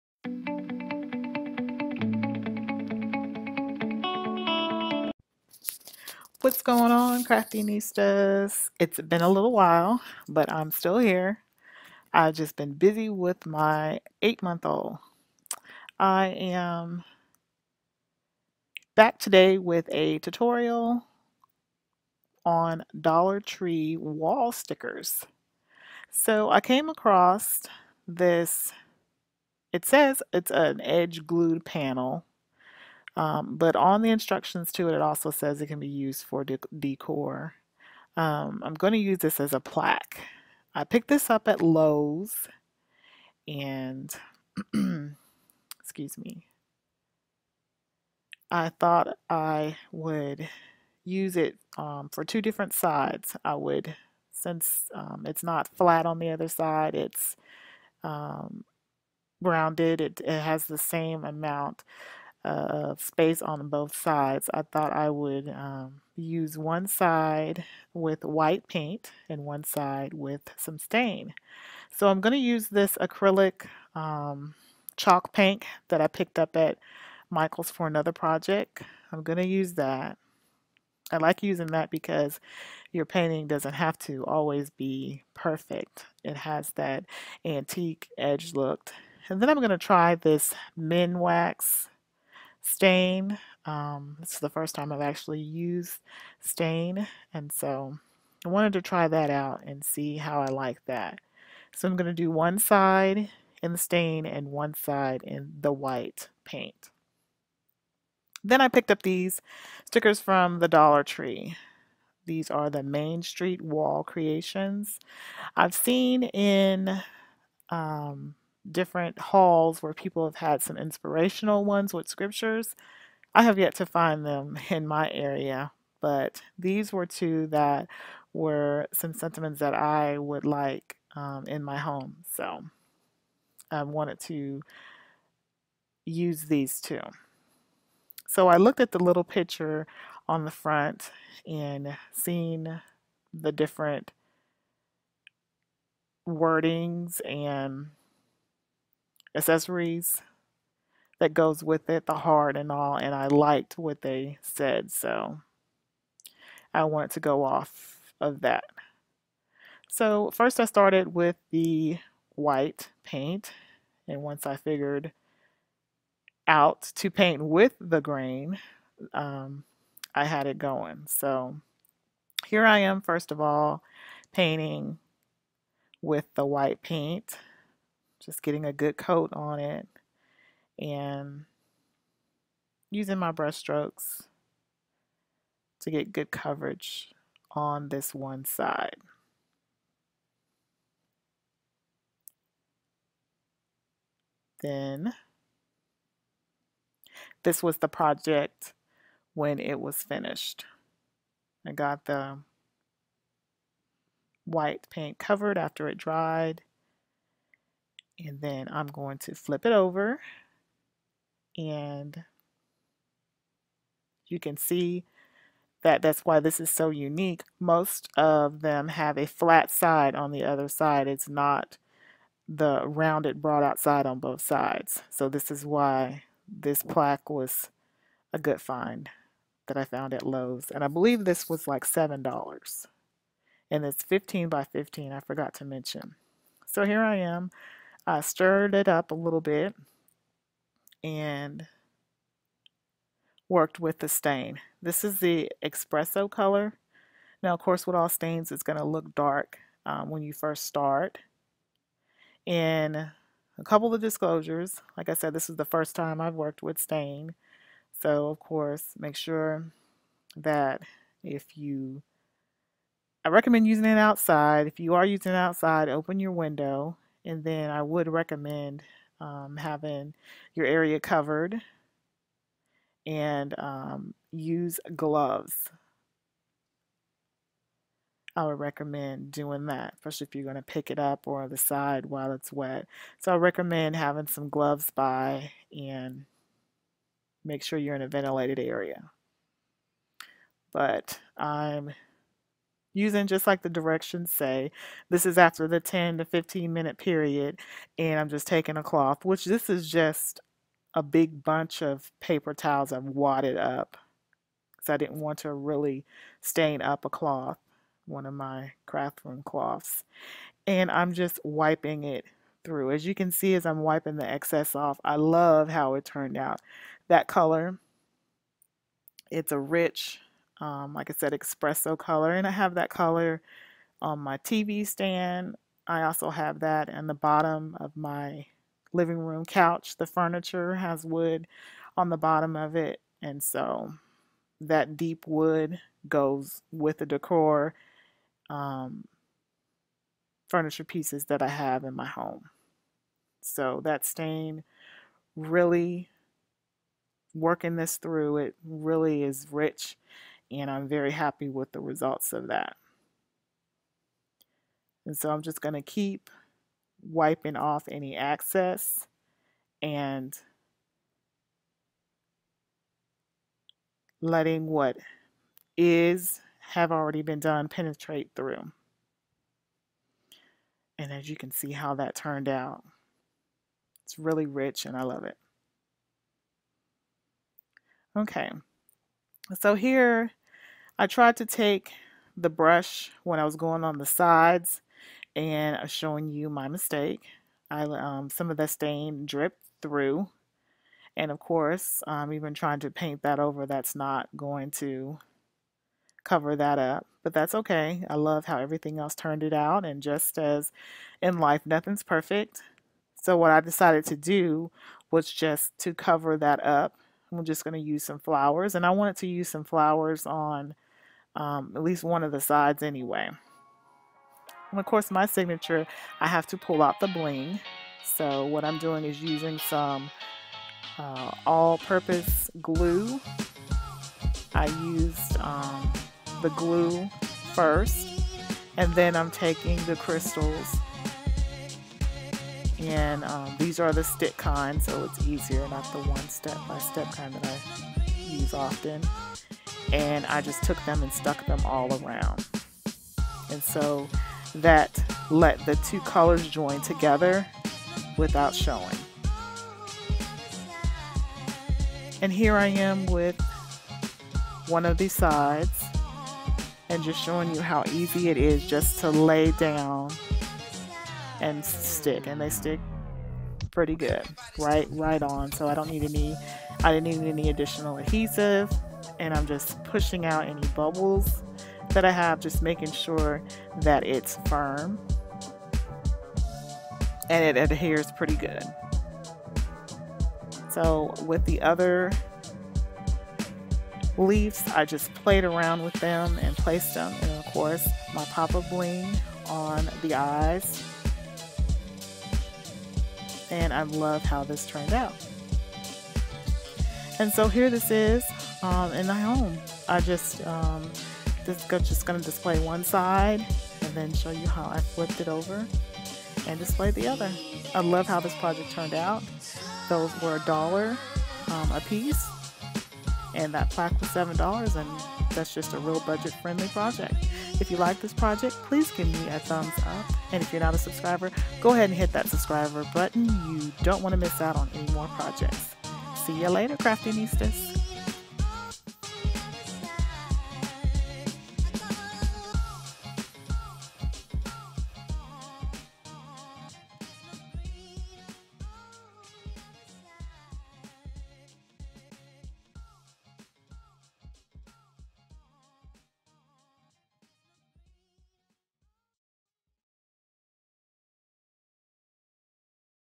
2 in 1 Plaque using Dollar Tree Wall Stickers
I purchased this two sided plaque at Lowes for $7 and also picked up two of the wall stickers for the decor isle at the Dollar Tree.  Music by Dj Quads